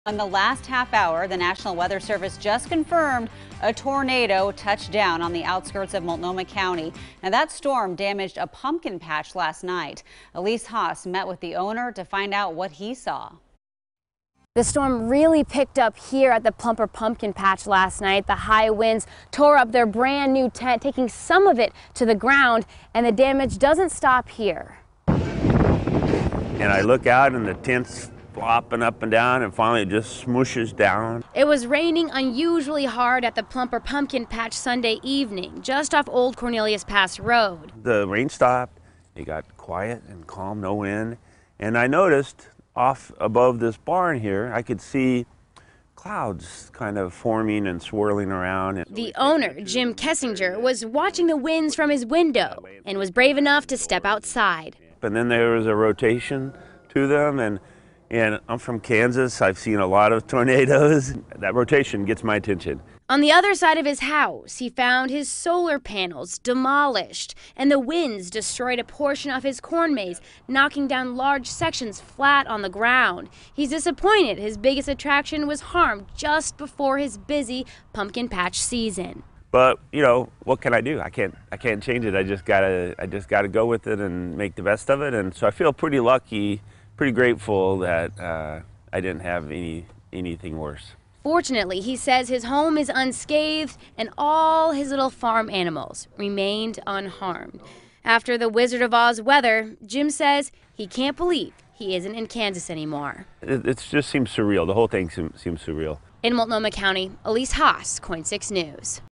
Pumpkin patch pulverized by twister
Jim Kessinger, the owner of the Plumper Pumpkin Patch, was watching the high winds from his window and was brave enough to step outside.